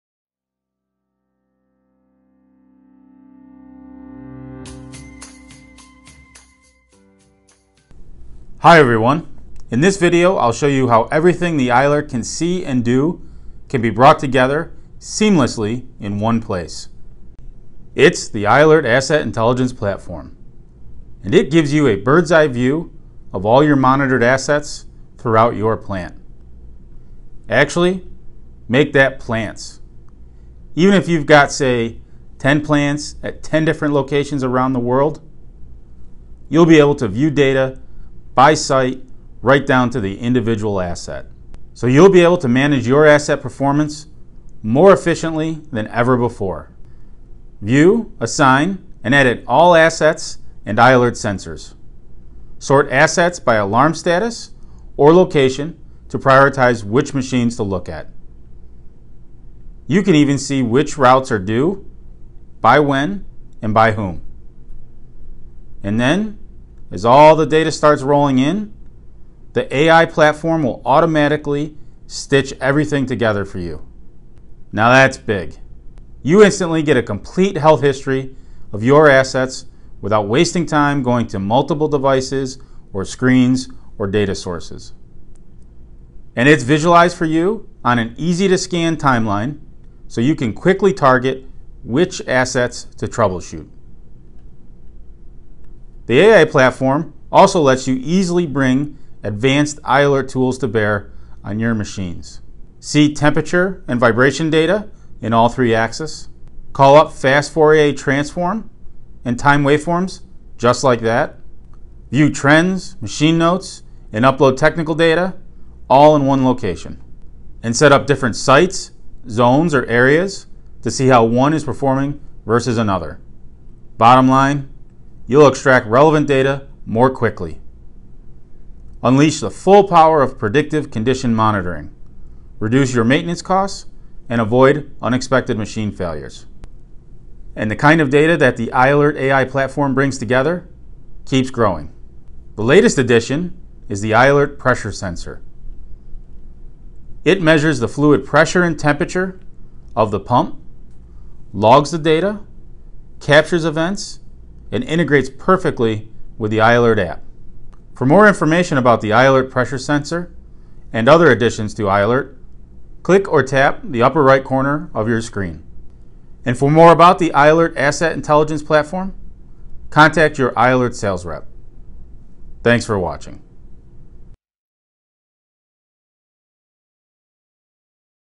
i-ALERT Ai Platform and the i-ALERT Compatible Pressure Sensor
Learn how the Ai Platform monitors all your i-ALERT devices and machines from a simple web interface and see how the i-ALERT compatible pressure sensor integrates by monitoring fluid conditions.